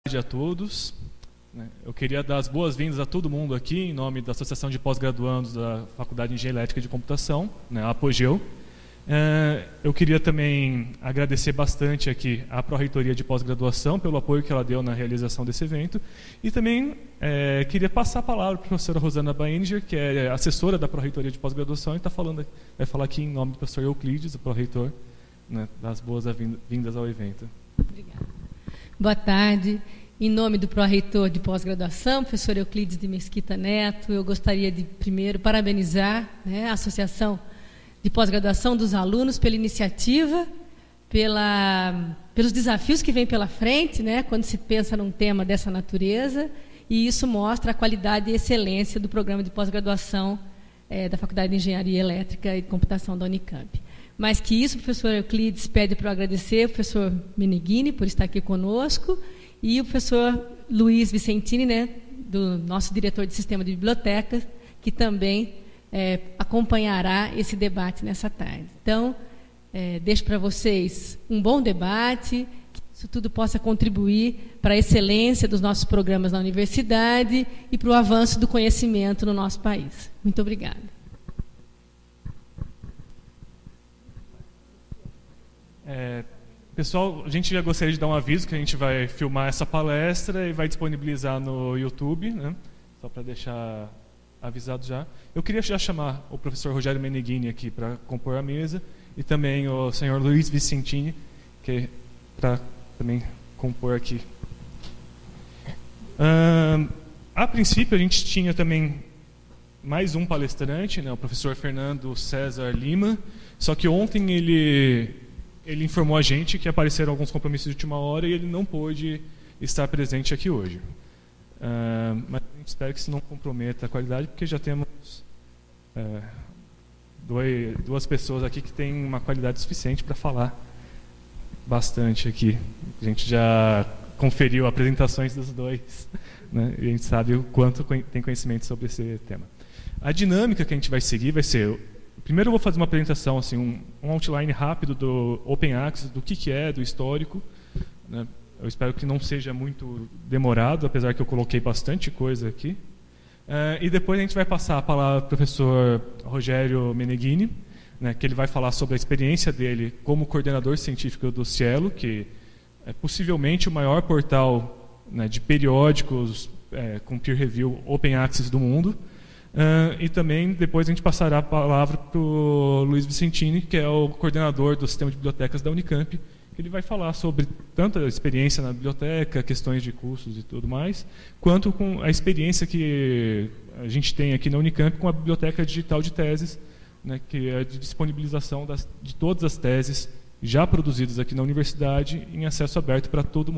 Debate sobre Open Access - Unicamp - Parte 1/5 - Abertura
Abertura do debate sobre acesso aberto a publicações científicas, realizado na Unicamp em 10 de outubro de 2012.

O evento foi organizado pela Associação de Pós-Graduandos da Faculdade de Engenharia Elétrica e de Computação da Unicamp (APOGEEU), com apoio da Pró-Reitoria de Pós-Graduação da universidade.